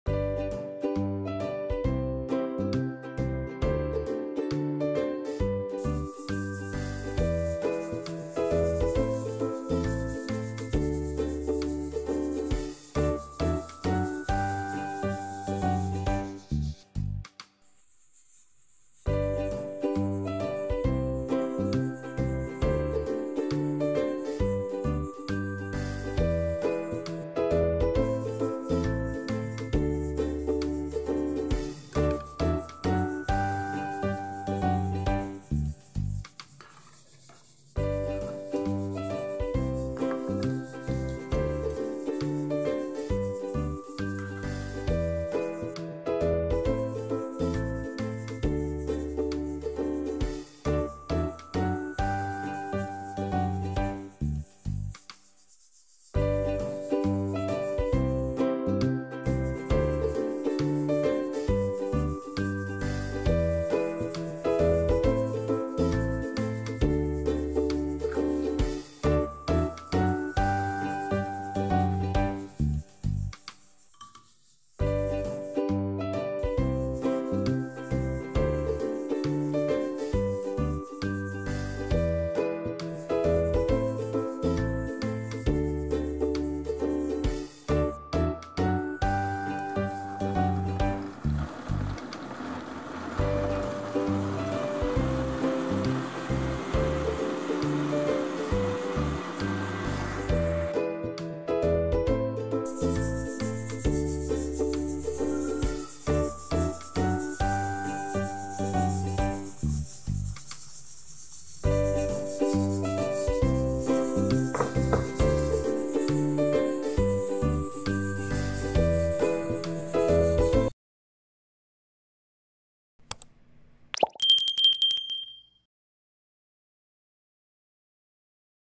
اسهل عصير طبيعي🥂🍹 ومنعش ممكن تحضريه في رمضان 2024 رائع وخاثر وغني بالفيتامينات
عصير طبيعي بالبرتقال